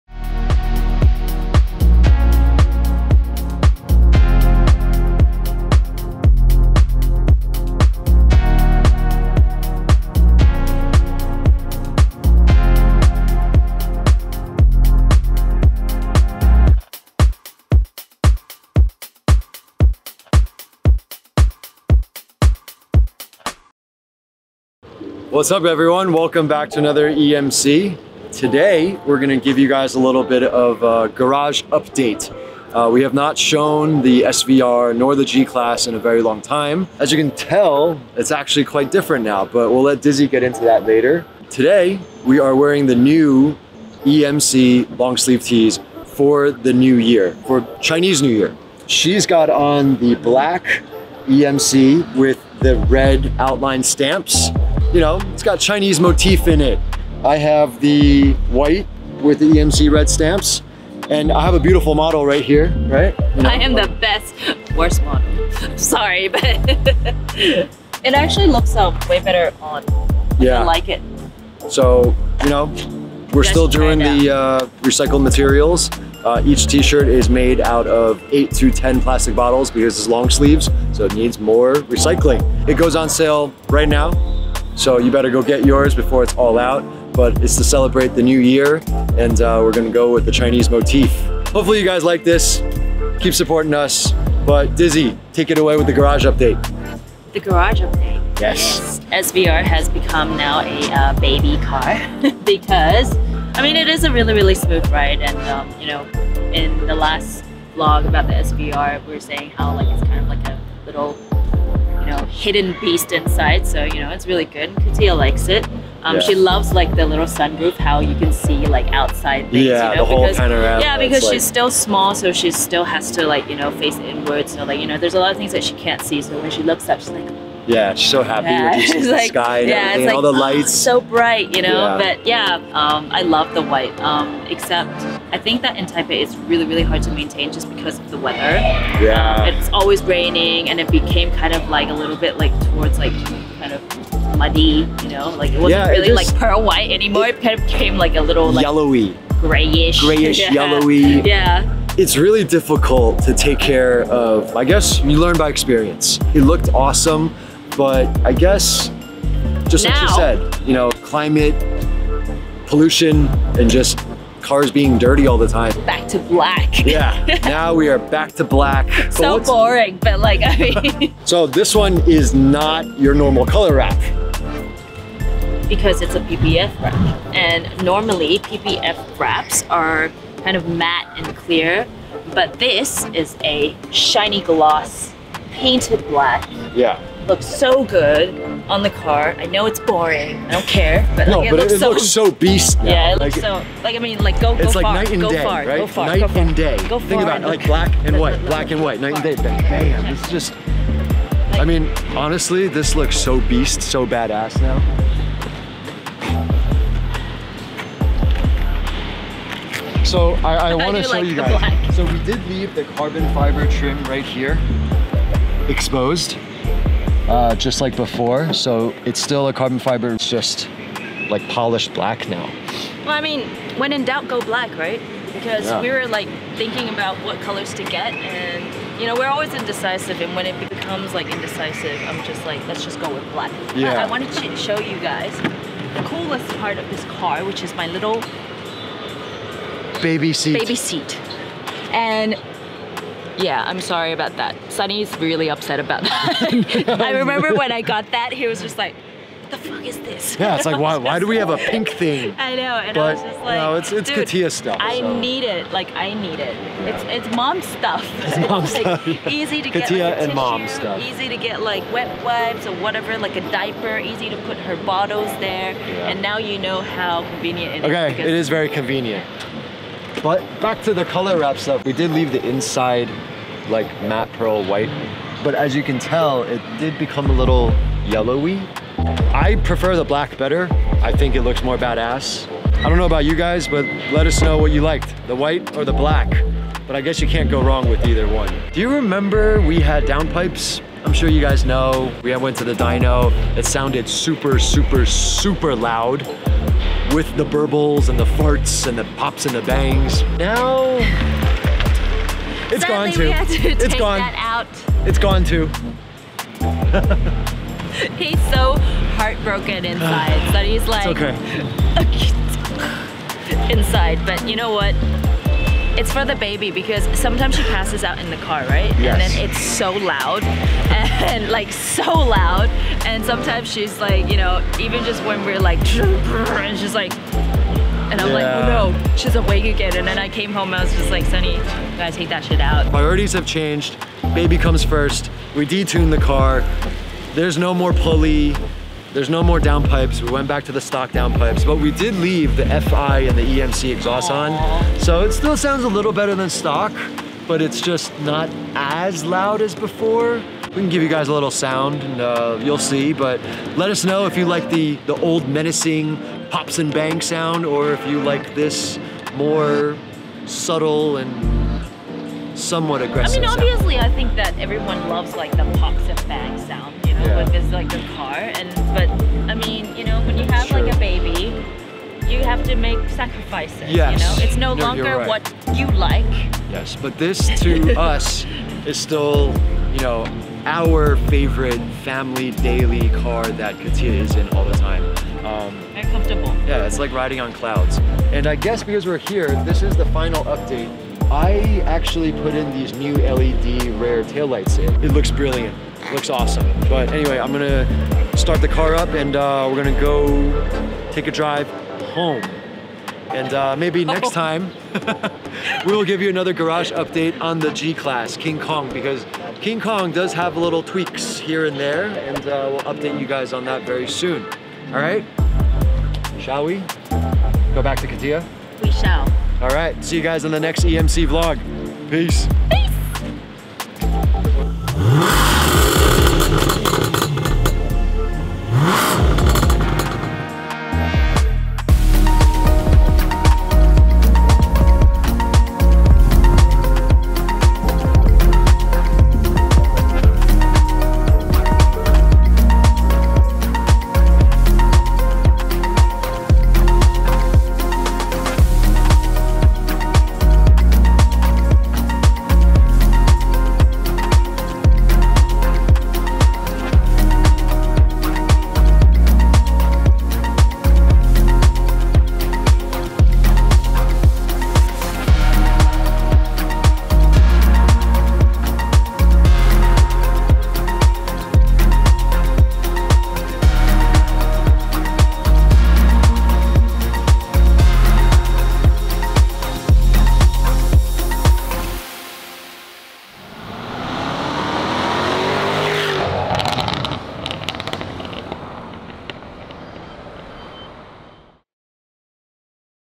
好久不見的SVR來看這陣子他變了多少 | LONG TIME NO SEE ...GUESS WHAT WE CHANGED TO THE SVR 《EMC Vlog Vol. 42》
有好一段時間我們沒有曝光這台SVR了 ，現在他是Dizzy的寶寶車，Katiya最愛的車。全家日出門就靠它了，又大又舒適。
這段時間也為了寶寶 做了一些更動，大家一起來看

在上集我們的抽獎活動 幸運的得主名單在下

郭大鐵
王元
黃聖傑

看到的請迅速私訊我們ig領取你的禮物！